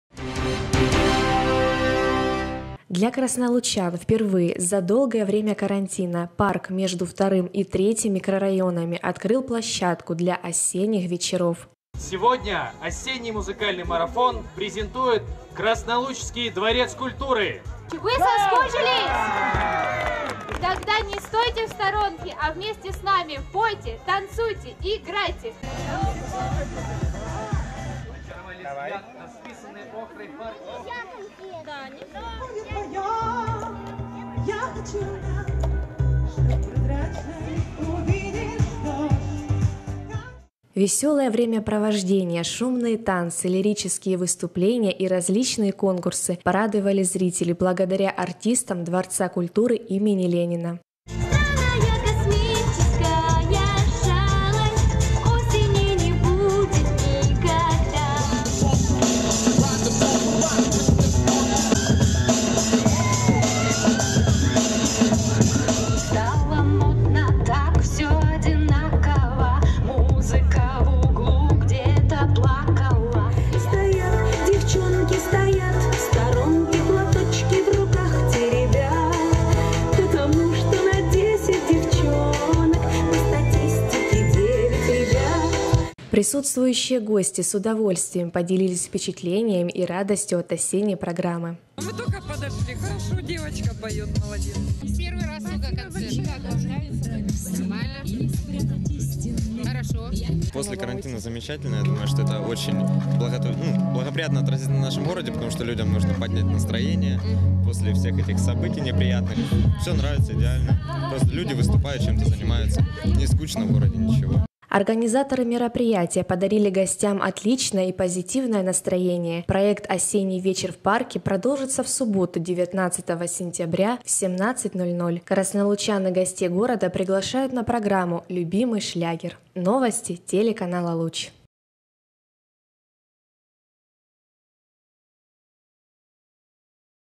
Первый "Осенний вечер" состоялся в Красном Луче
В субботу, 12 сентября, по инициативе администрации города Красный Луч в парковой зоне между вторым и третьим микрорайонами прошла концертная программа «Осенний вечер в парке».